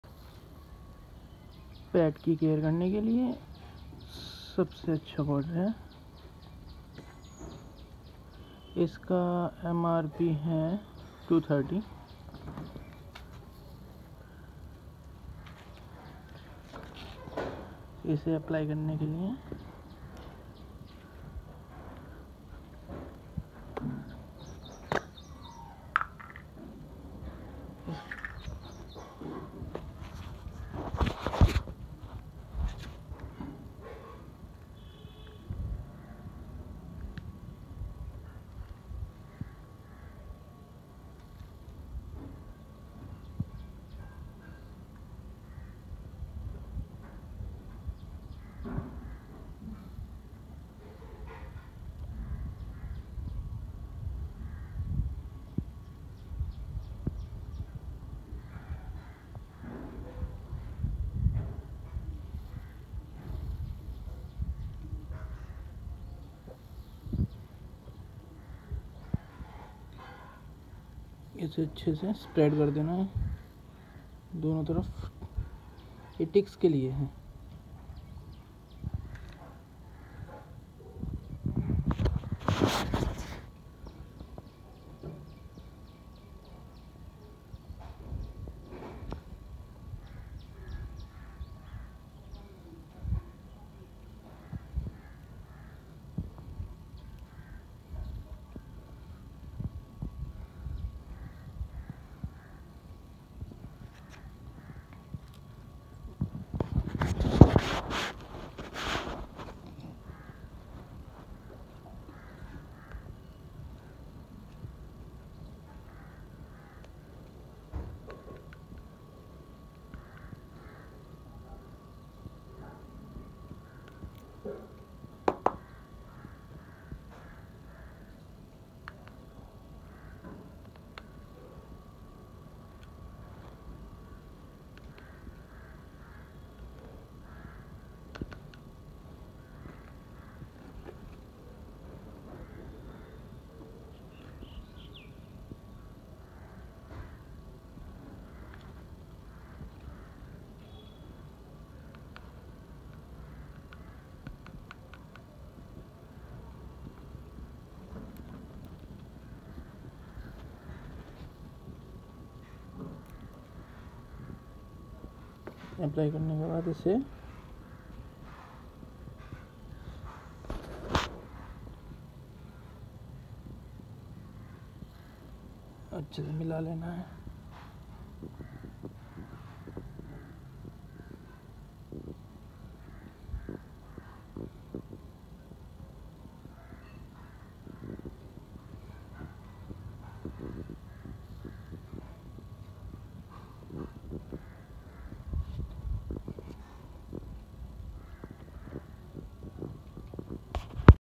Ticks remove trick
Ticks are most commonly found around a dog's head, neck, ear and feet. A tick feels like a small bump on your pet's skin. And removing trick…🐕🐕🐕🐕🐕🐕